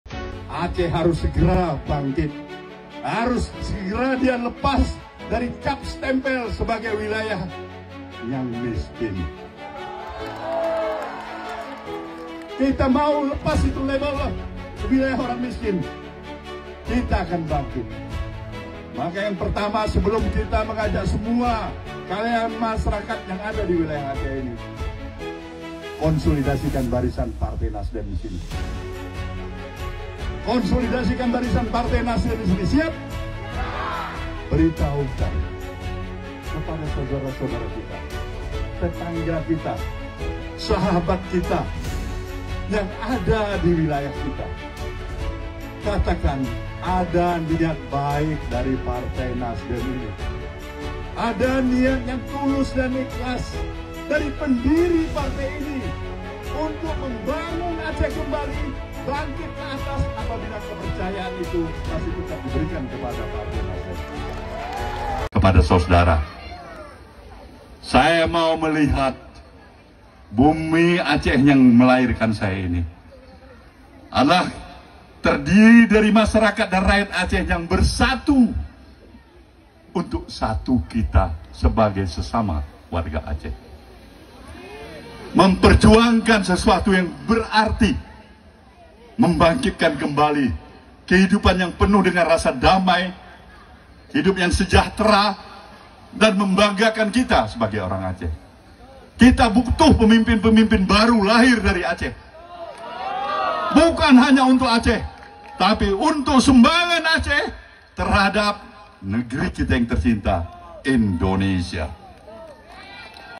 Pembukaan yang Menggemparkan: Pesan Menohok Surya Paloh untuk Aceh!
Surya Paloh mengawali pembicaraannya dengan pesan menohok yang membangkitkan semangat masyarakat Aceh. Temukan makna mendalam di balik kata-kata kuat ini yang diungkapkan oleh Surya Paloh untuk menginspirasi dan menggerakkan hati para pendengar.